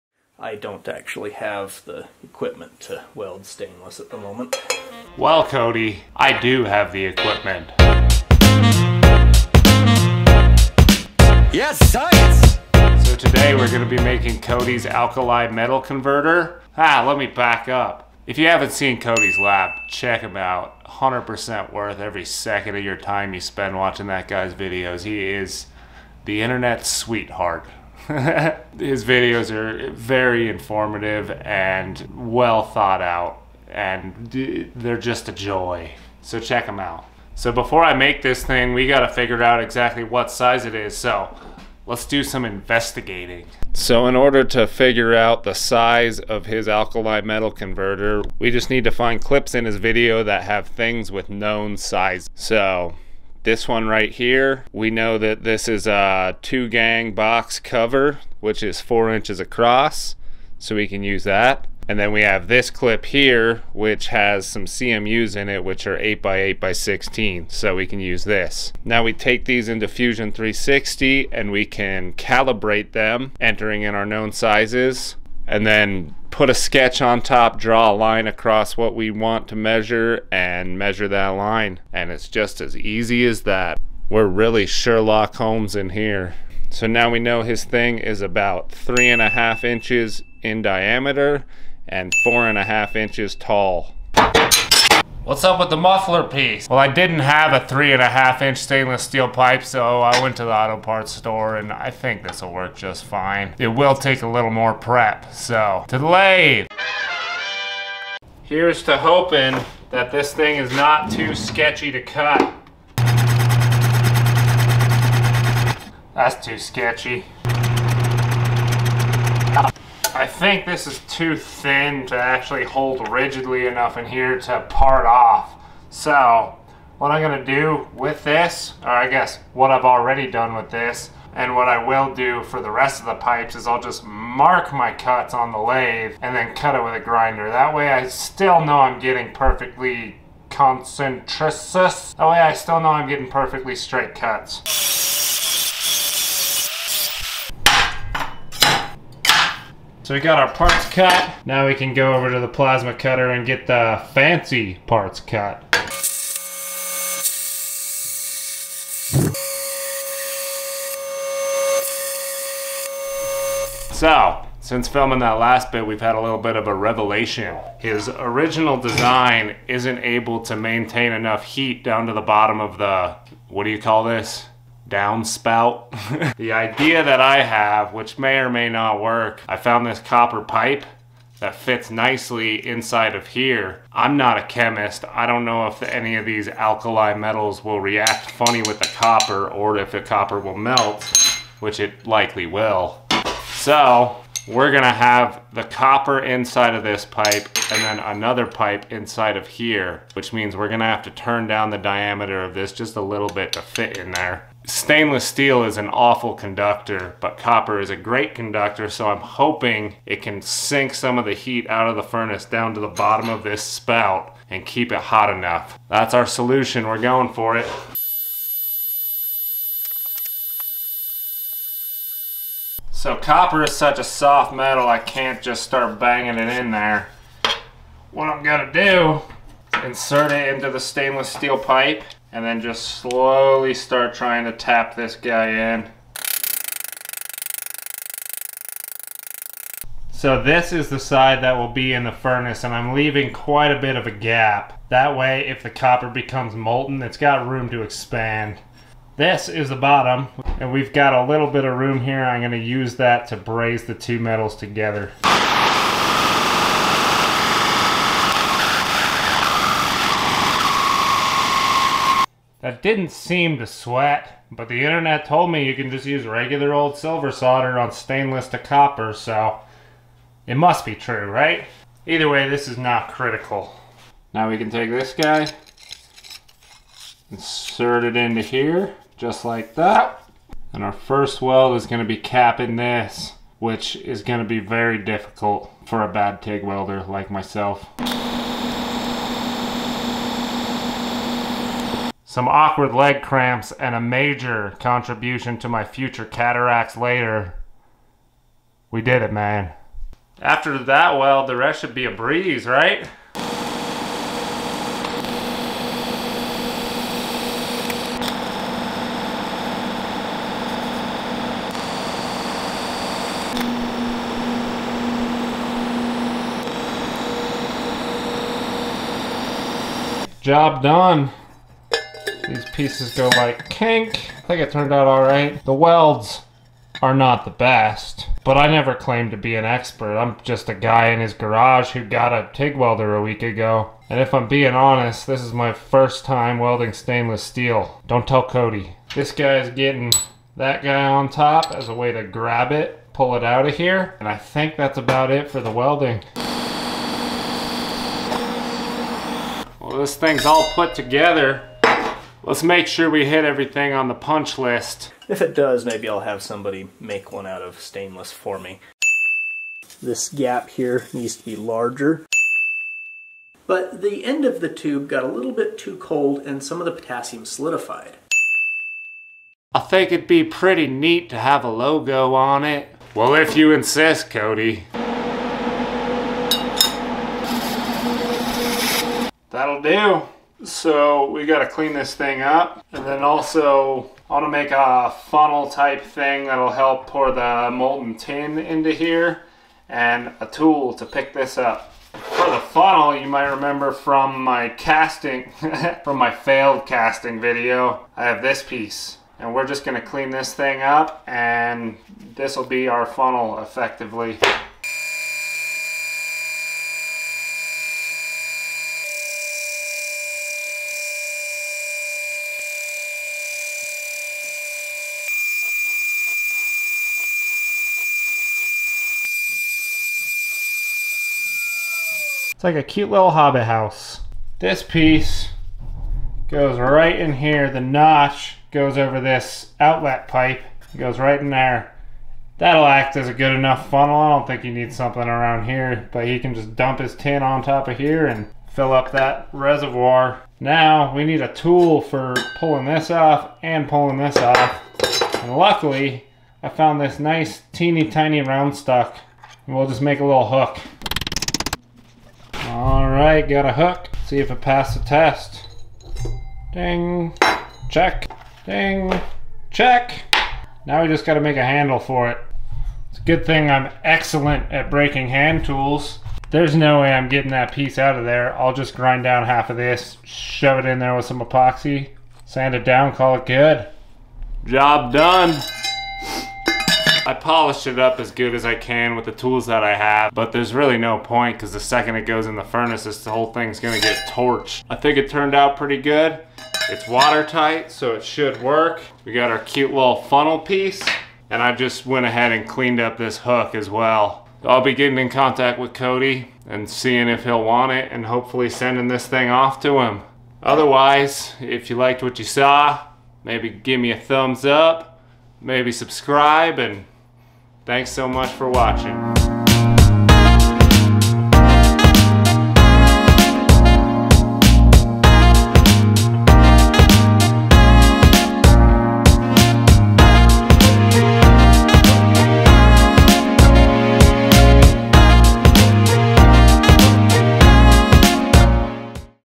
A Gift For Cody'sLab
Making a new alkali metal converter out of stainless for Cody.
I actually shot this video a while ago, I wanted to wait until I got in contact with Cody until I posted it, but as it turns out, he's a hard man to get in contact with! So, if any of you know how to contaxct him, let me know! (or let him know haha)